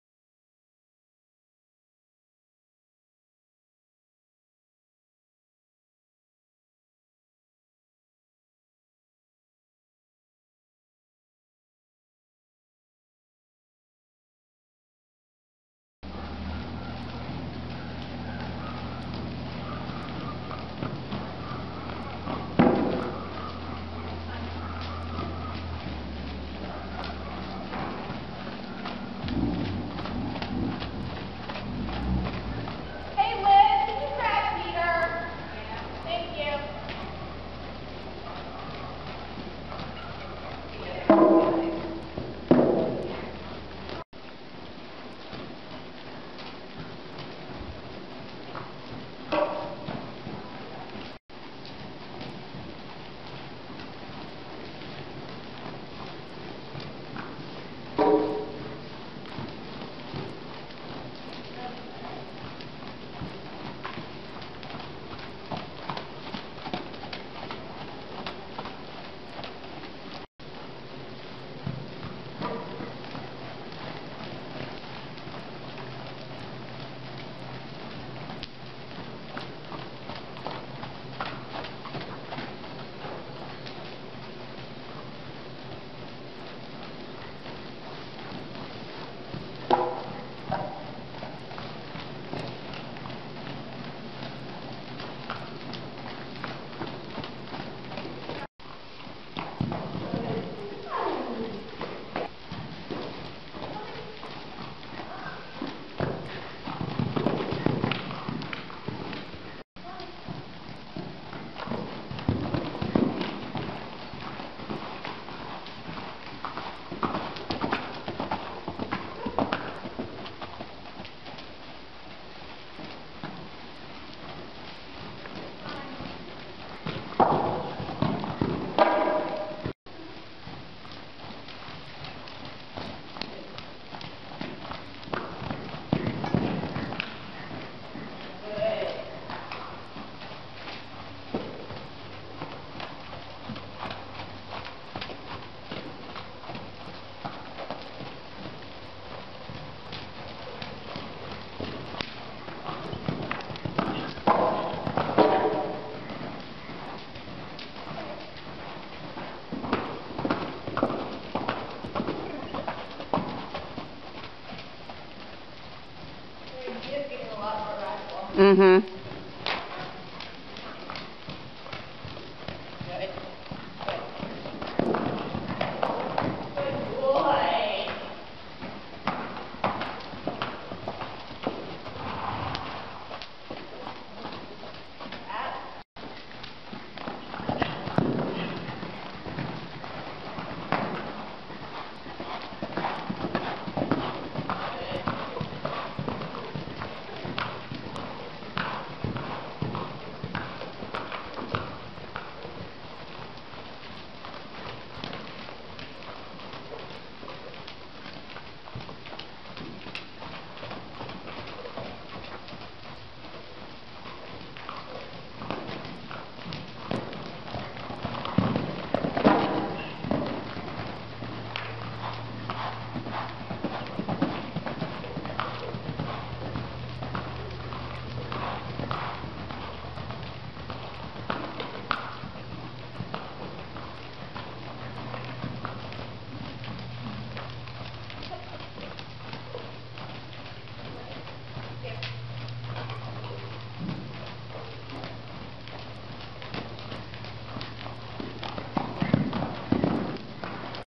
Doc's Option Intro to Jumping
TB gelding, 17-1 hands, 13 years, learning to jump. Shown in dressage. Just started over fences.